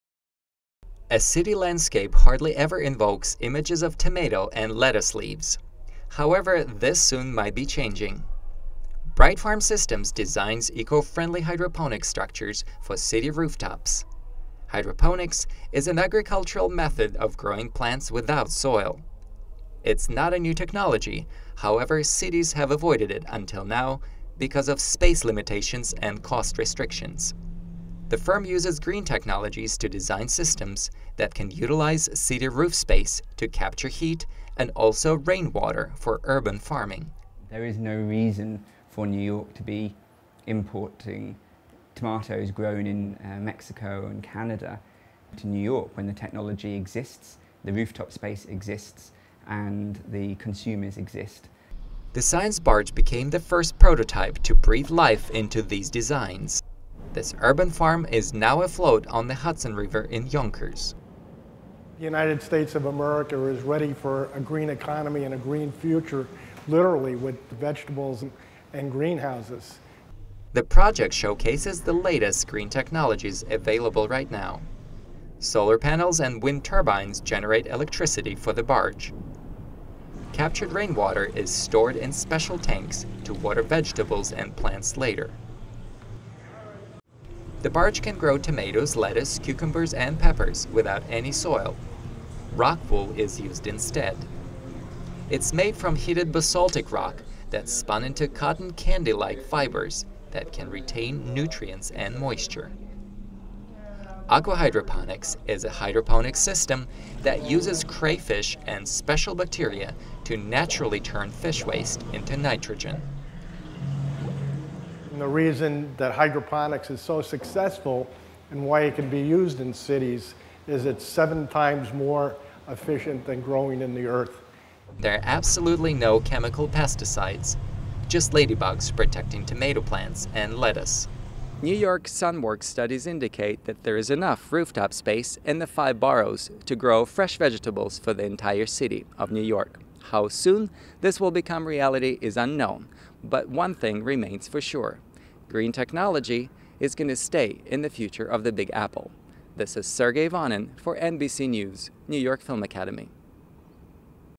urban farming in NYC and the green barge
BrightFarms Systems designs hydroponic systems for urban farming with minimum carbon footprint. The Green Barge, a showcase of how these green technologies can be applied in a city, is currently docked in Yonkers, NY